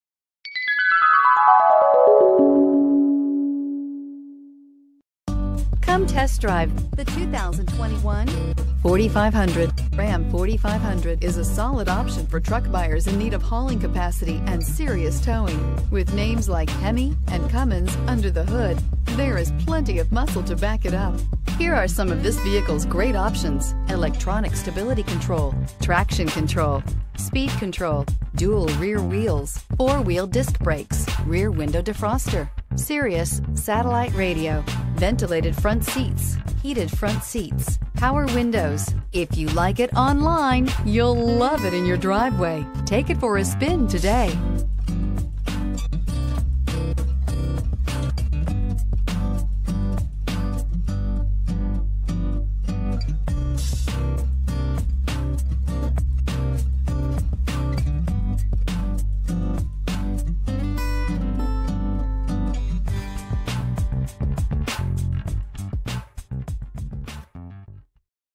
2021 Ram 4500 Chassis Cab Cleveland GA R5094
2021 Ram 4500 Chassis Cab Laramie

Looking for the right crew cab chassis-cab? Today could be your lucky day. this 2021 Ram 4500 Chassis Cab could be yours.